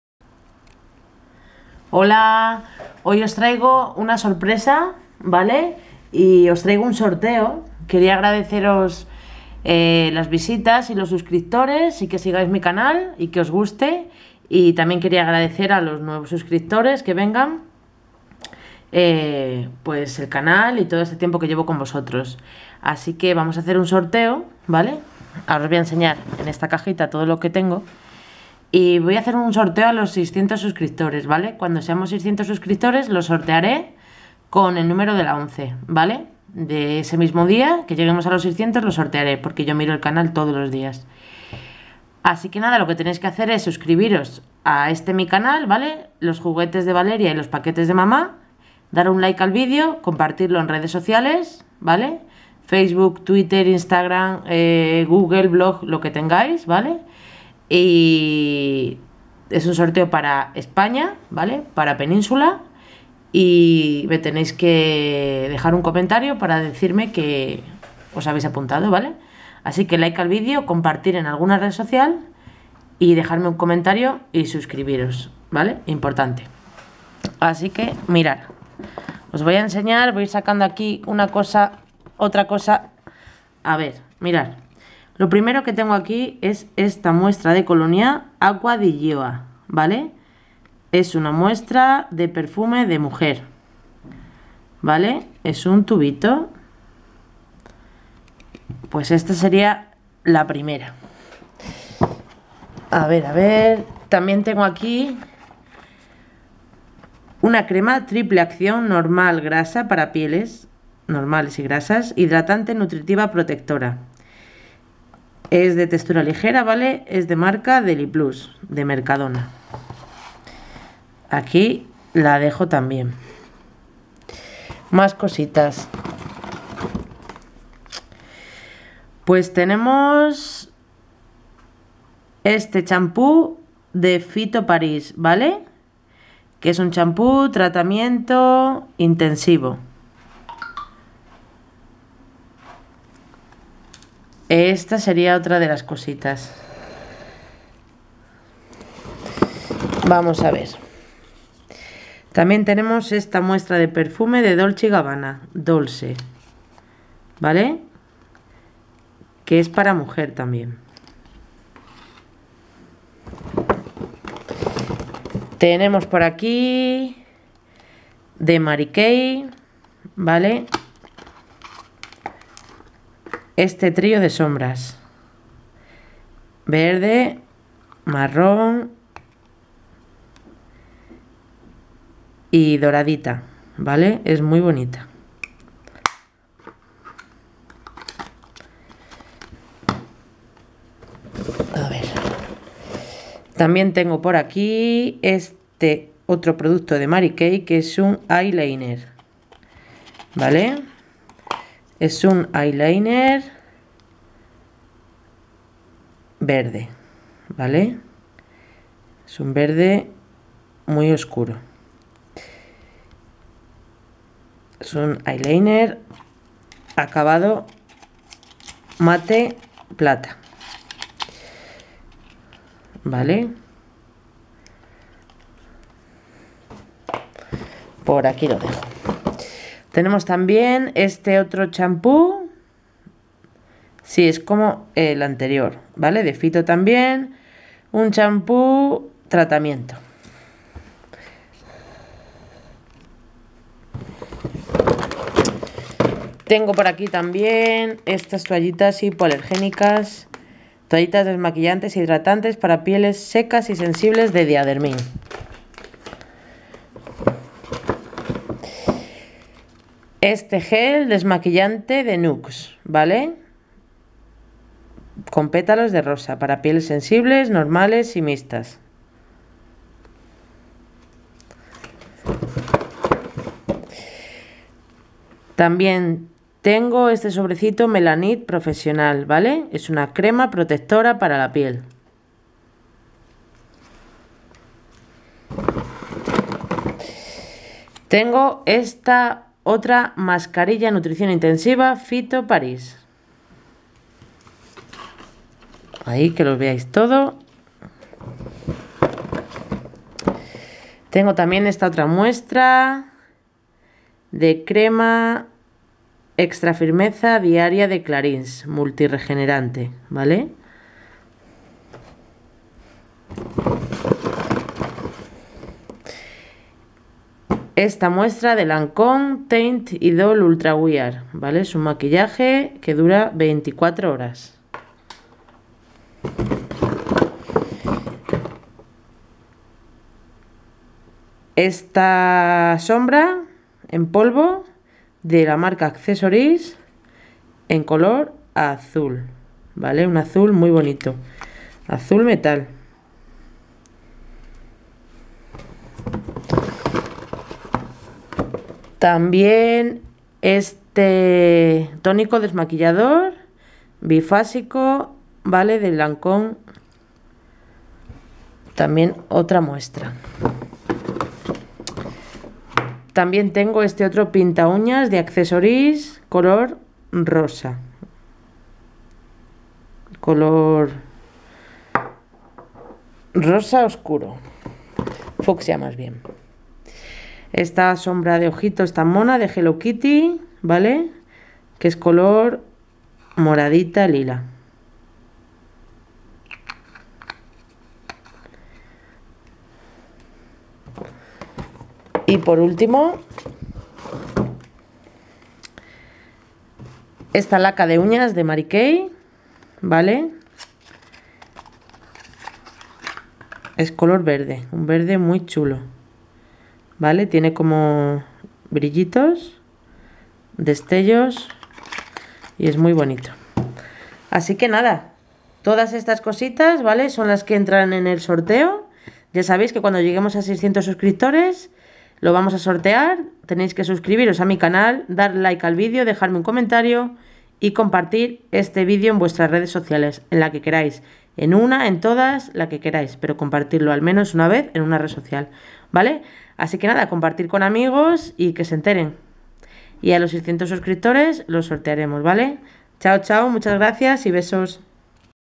SORTEO CERRADO. GANADORA: Verónica G. Couso
Hola. Quería agradeceros vuestra presencia en mi canal, vuestros likes, vuestros compartidos y vuestras suscripciones con este sorteo. Espero que os guste. Sobre todo gracias por estar ahí.
*Cuando seamos 600 suscriptores en mi canal lo realizare con el sorteo de la once de ese mismo día.
**REQUISITOS:
1. Dar like al vídeo
2. Compartirlo en una red social en modo público. (En una, dos, tres o las que queráis, pero al menos una).
3. Y mas importante estar suscritos a mi canal.
4. Dejarme un comentario si queréis participar y decirme o contarme lo que queráis.
Os iré asignando número por orden cronológico.
**Sorteo solo válido para España. Península. (Cuando seamos mas suscritos podré enviarlos fuera de la península, perdonarme).
Muchas gracias. Eso es todo.